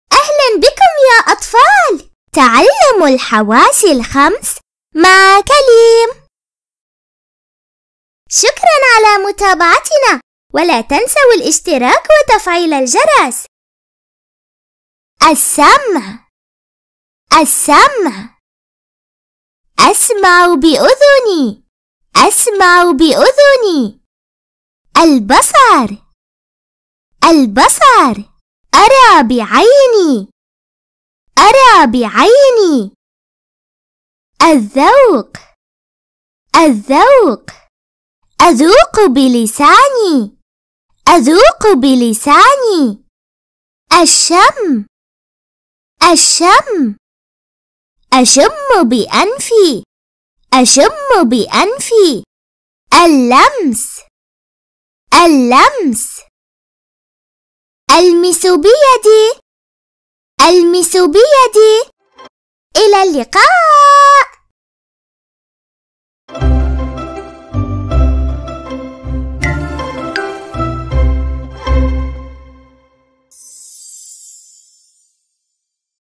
الحواس الخمس Les 5 (cinq) sens en arabe
تعلم الحواس الخمس مع كليم
Apprendre les 5 cinq sens en arabe (français et anglais) avec kalim.